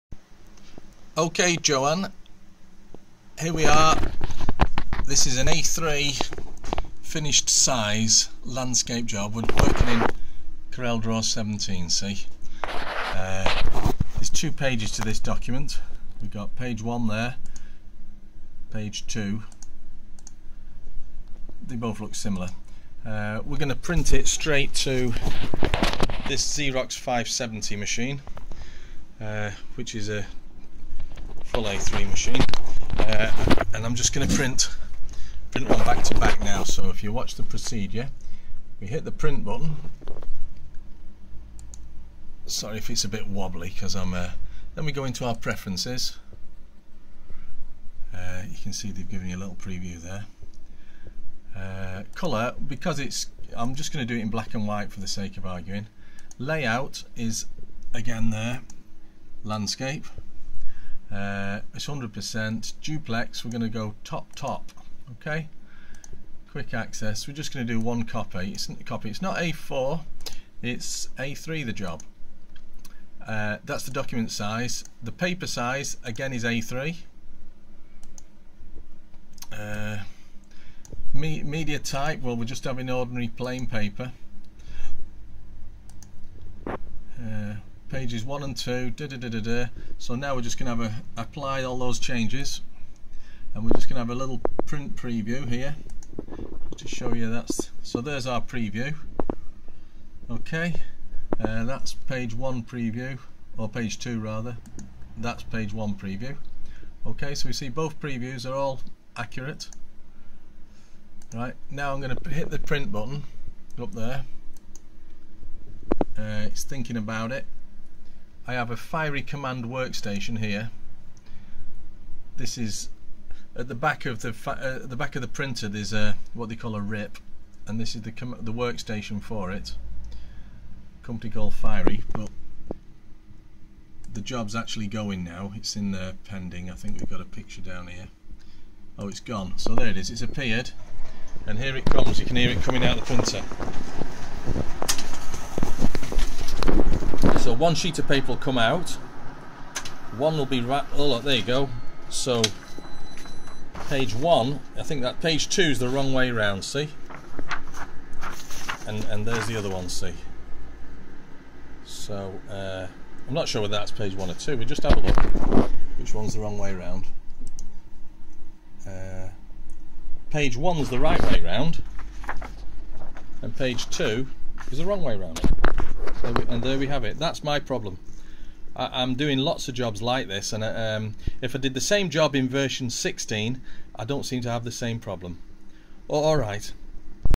Duplex printing problems corel draw 17
Here we have a short video showing the problems i have printing duplex from corel draw 17 straight through to my xerox 570 printer with fiery rip